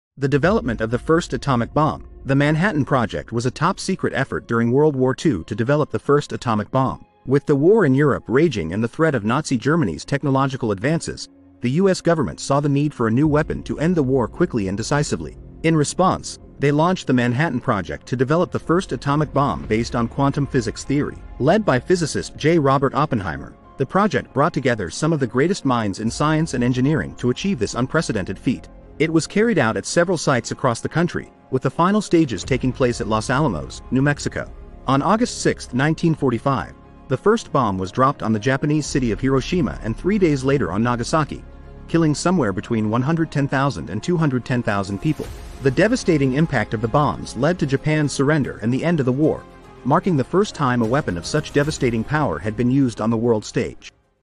The Manhattan Project: The Development of the Atomic Bomb
The Manhattan Project was a top-secret effort during World War II to develop the first atomic bomb.

Led by physicist J. Robert Oppenheimer, the project brought together some of the greatest minds in science and engineering to achieve this unprecedented feat.